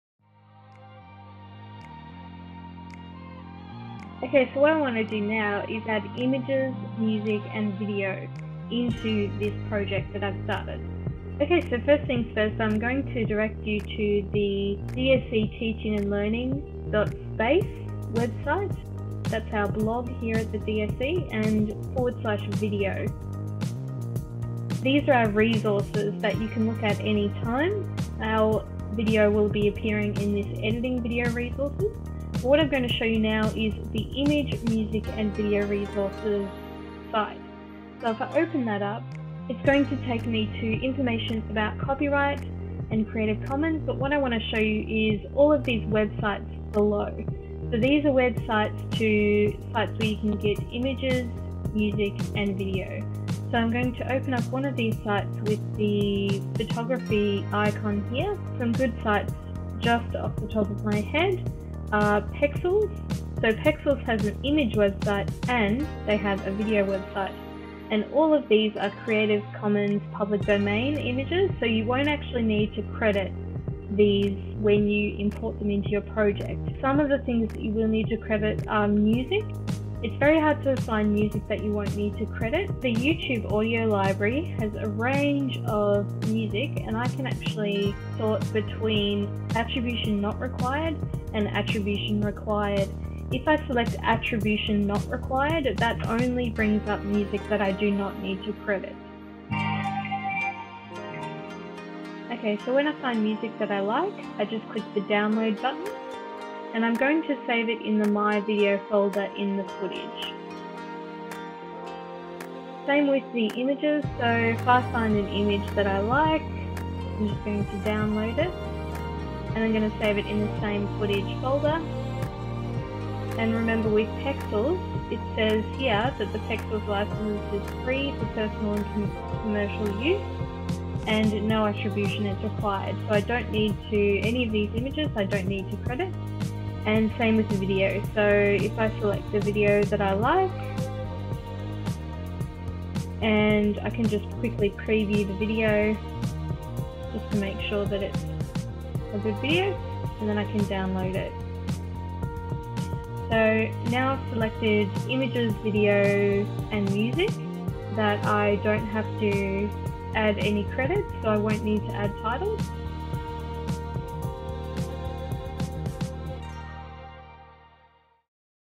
Sourcing images, music and videos
This video describes how to source creative commons images, music and videos.

view the list of websites.